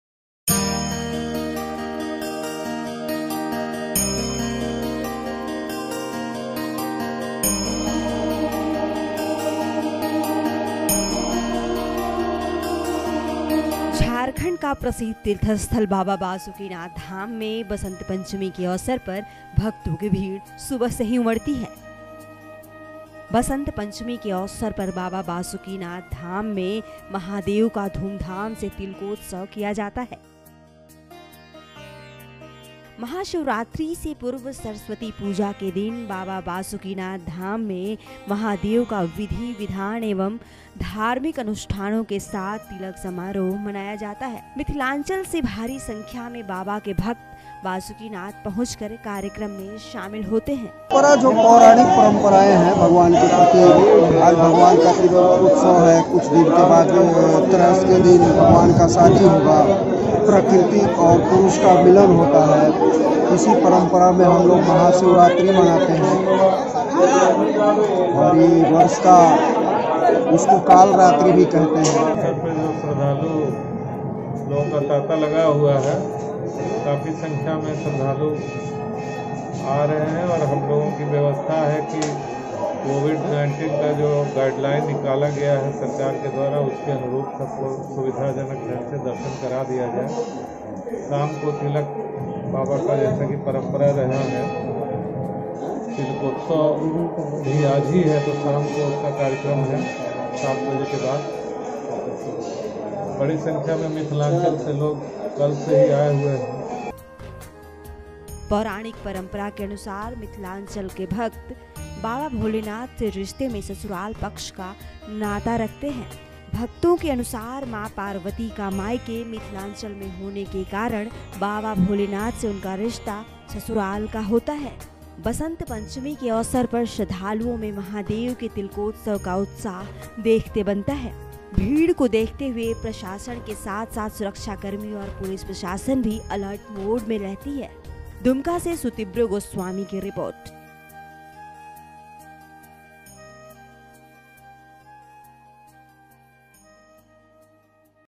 महादेव के तिलकोत्सव में पहुंचे मिथिलांचल वासी
दुमका (DUMKA) - झारखंड के प्रसिद्ध तीर्थ स्थल बाबा बासुकीनाथ धाम में आज बसंत पंचमी के अवसर पर शिव भक्तों की भीड़ सुबह से ही उमड़ी रही है. मंदिर में सुबह से ही शिव भक्तों का तातां लगा हुआ है. बता दें कि आज बसंत पंचमी के अवसर पर बाबा बासुकीनाथ धाम में महादेव का धूमधाम से  तिलक किया जाएगा. महाशिवरात्रि से पूर्व सरस्वती पूजा के दिन बाबा बासुकीनाथ में महादेव का विधि विधान एवं धार्मिक अनुष्ठानों के साथ तिलक समारोह मनाया जाएगा.